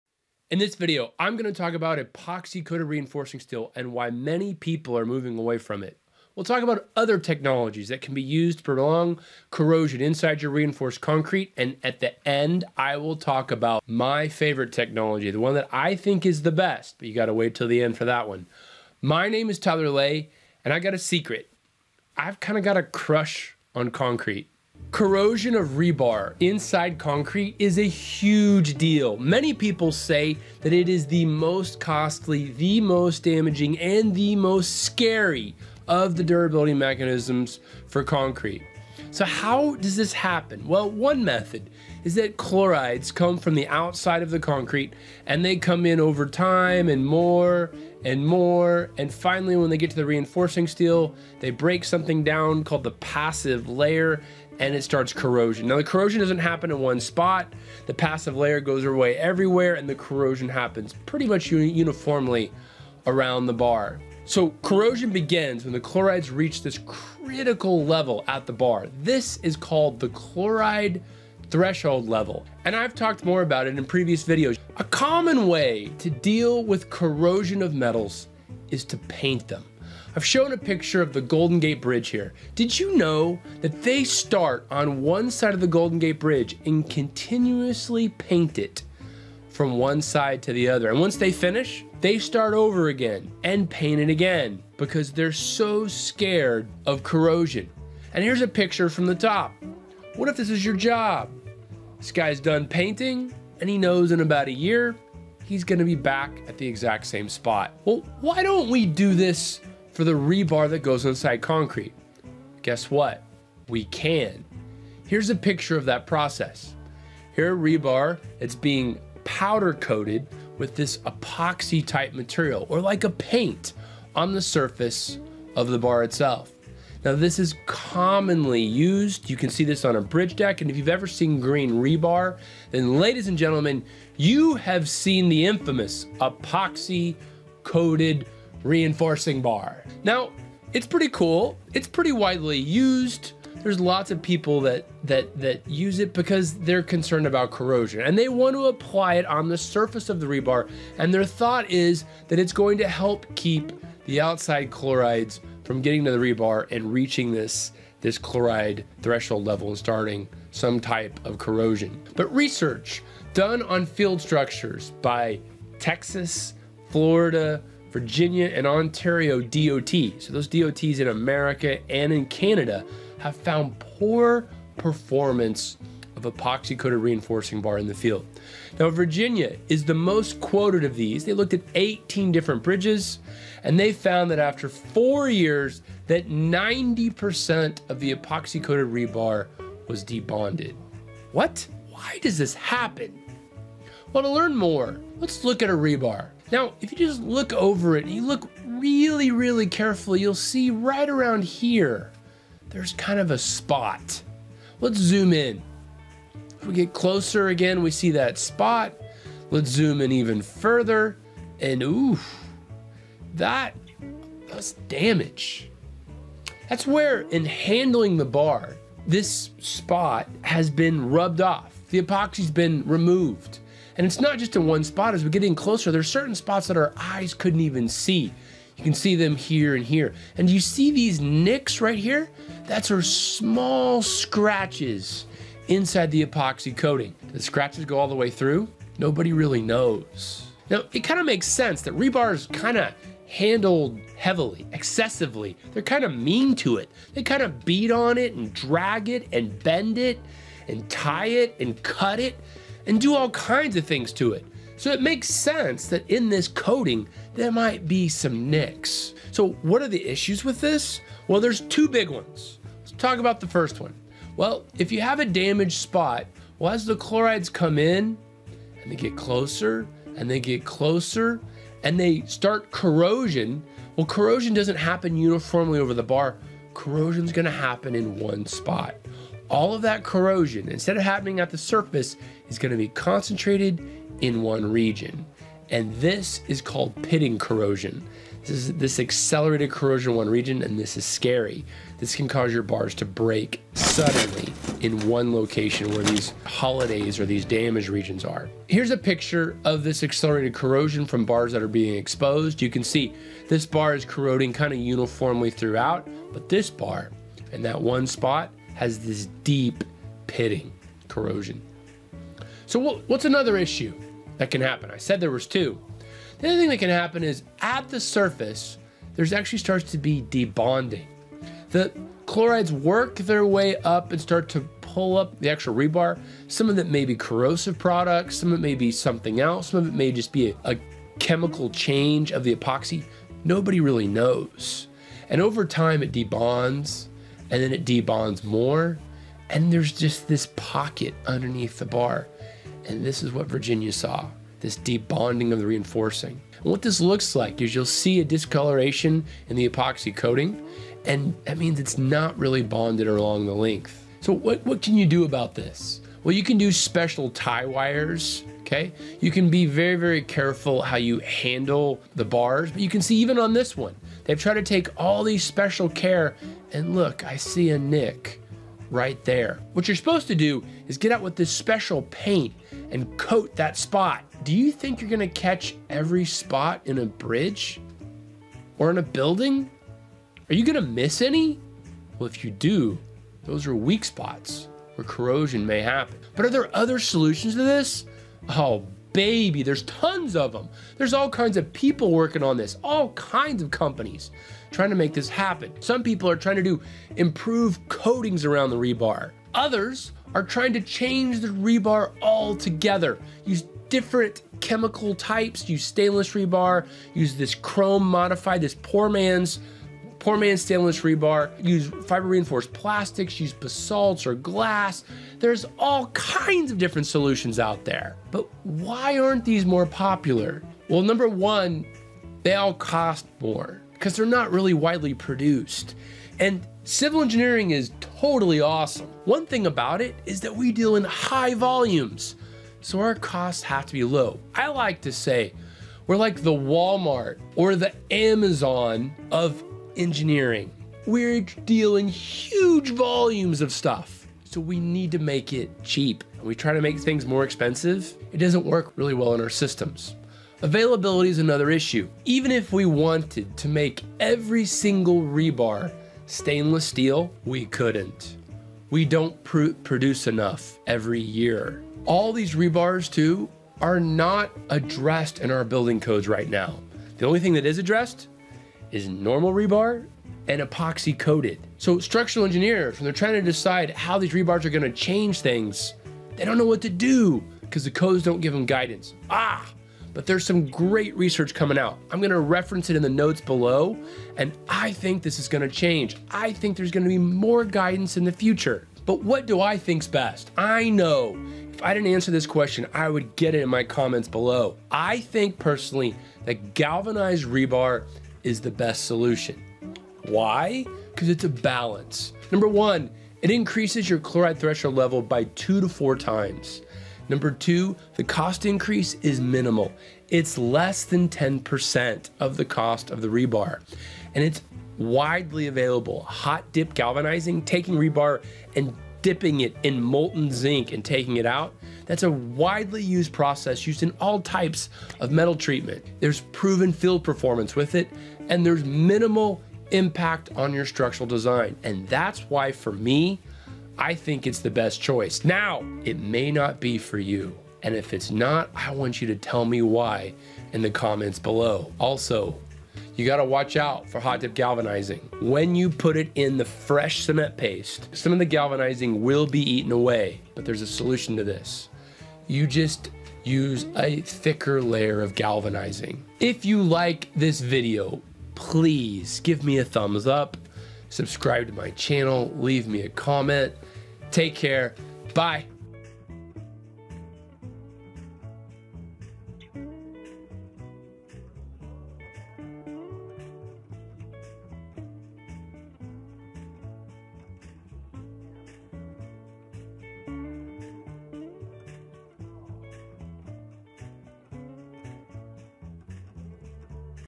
What is epoxy coated rebar and why is it being banned?
This video explains why epoxy coated rebar is used in concrete.  It also discusses the challenges with the use of epoxy coated reinforcing bars.  The video will discuss why many states are moving away from the use of epoxy coated reinforcing bars.  Then the video talks about other types of reinforcing bars and how they can be used to stop corrosion within concrete.

Thanks to Hailey Goodale!  She edited this video and is my Director of Awesome.